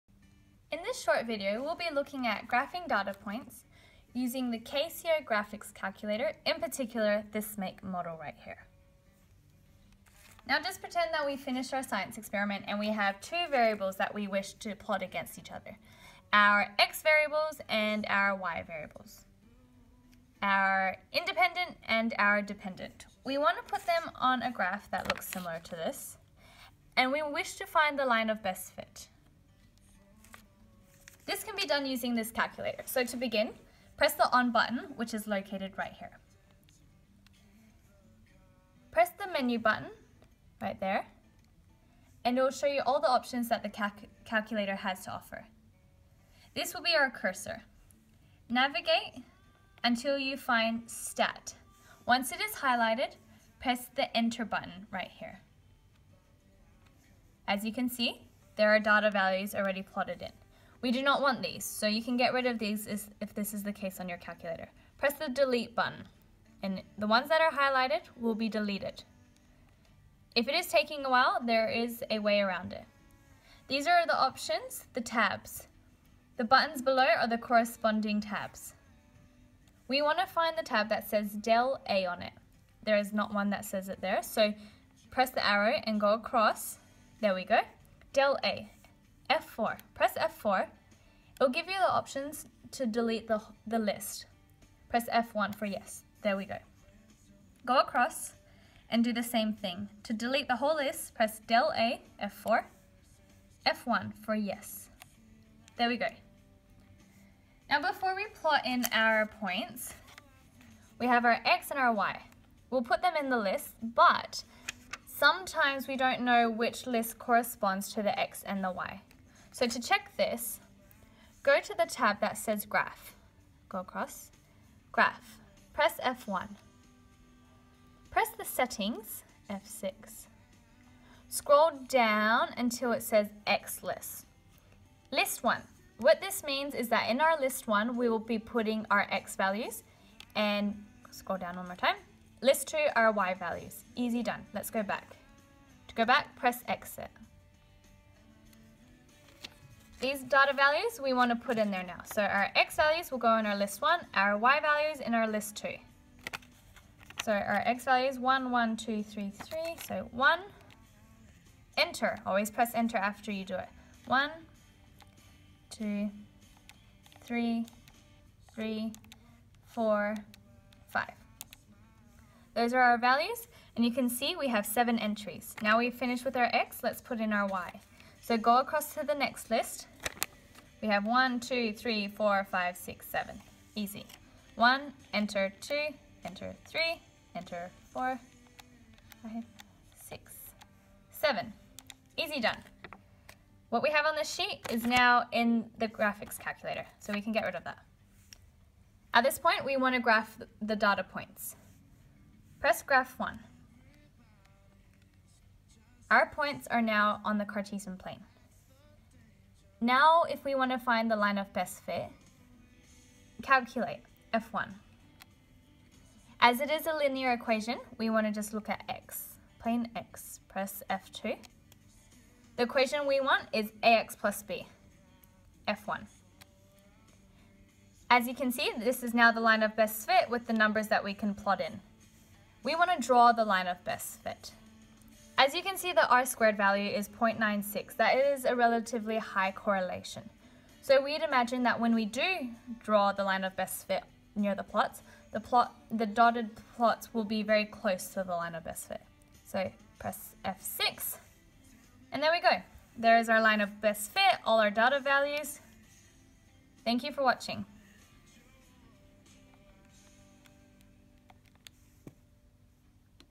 Drawing the line of best fit using the Casio fx-9860G AU graphics calculator
Tutorial on how to graph data points and calculate the line of best fit on the Casio fx-9860G AU graphics calculator.